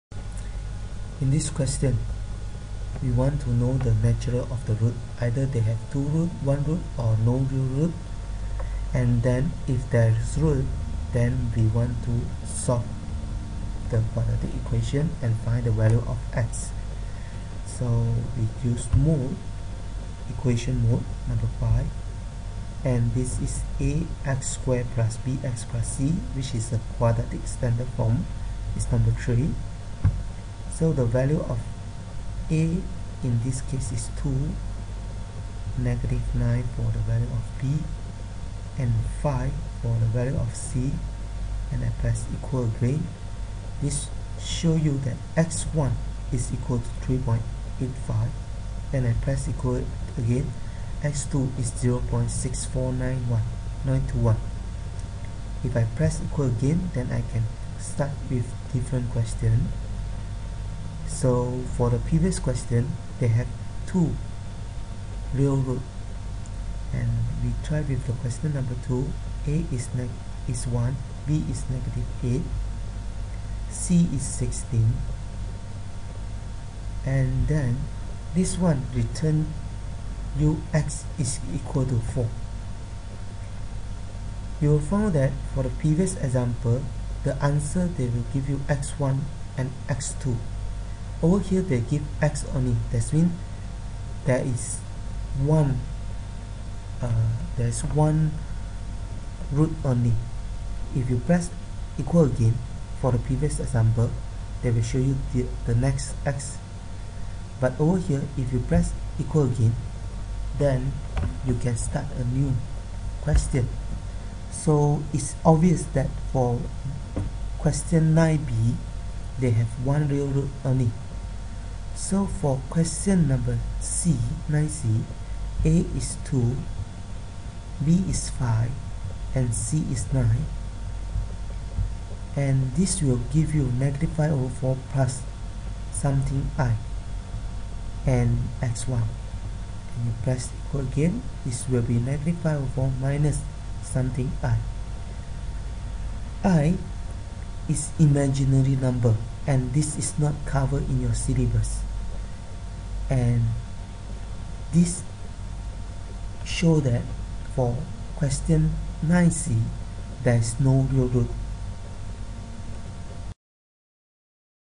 5. Quadratic Equation.avi
Use Casio fx-570ES Calculator to answer Quadratic Equation question.